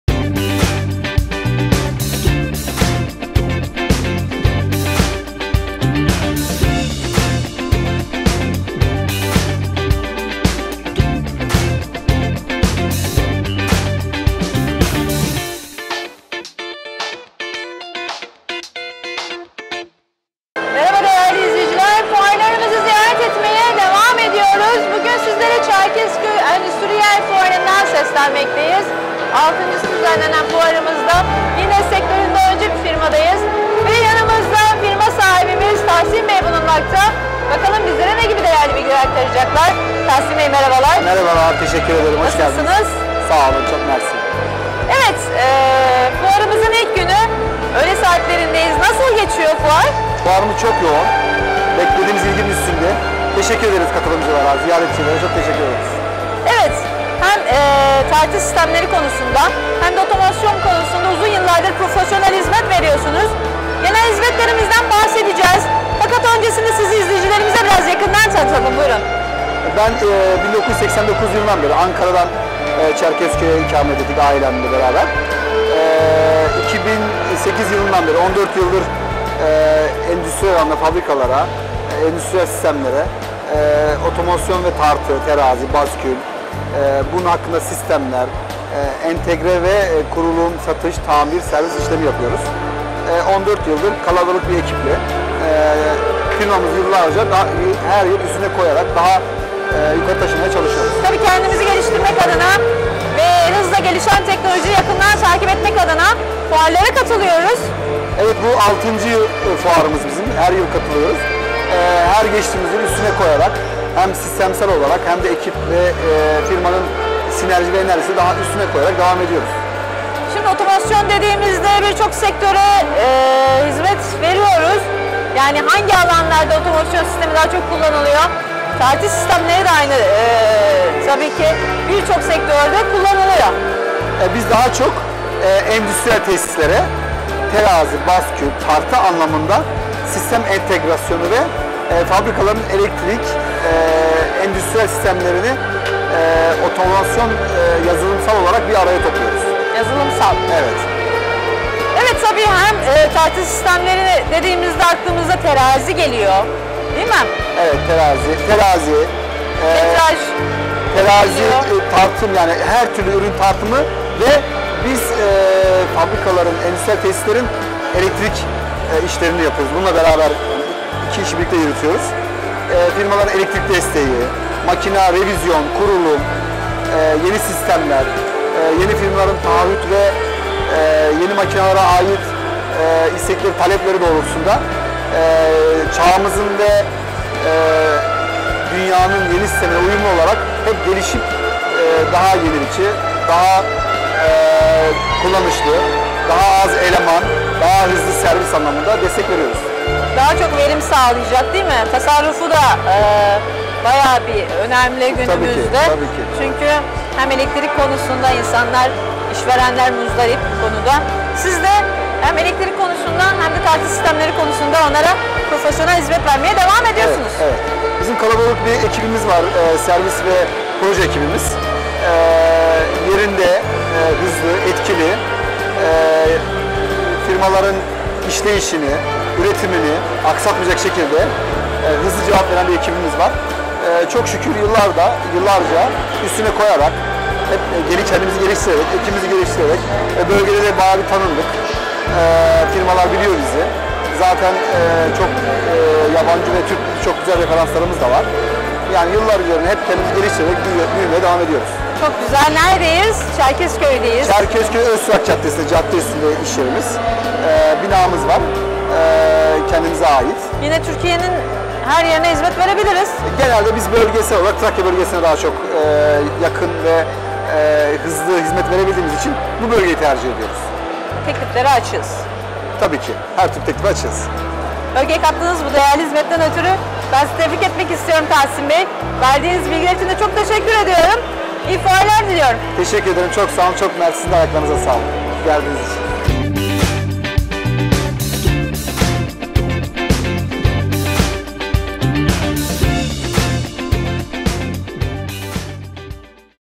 DEMKON OTOMASYON VE TARTI SİSTEMLERİ - ÇEF FUARI
DEMKON OTOMASYON VE TARTI SİSTEMLER - ÇEF FUARI

Tekirdağ ili Çerkezköy İlçesinde faaliyette bulunan DEMKON OTOMASYON VE TARTI SİSTEMLER Firma Sahibi Sn. Tahsin DEMİR, başarı ve faaliyetlerini ÇEF FUARI firma standında, Fuar TV muhabirimiz Zuhal TARIMER' e anlattı.